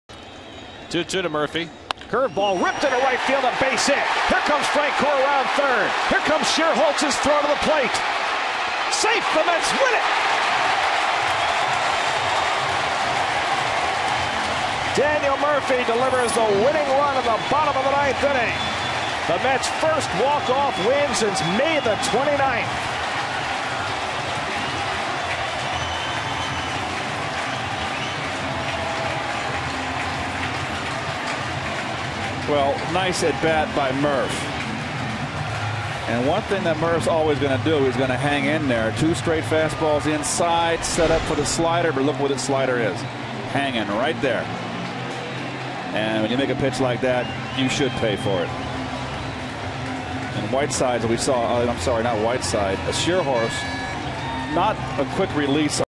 Francoeur beats the tag and the Mets walk off
8/16/09: Daniel Murphy singles to right, and Jeff Francoeur beats the tag to hand the Mets the 3-2 walk-off win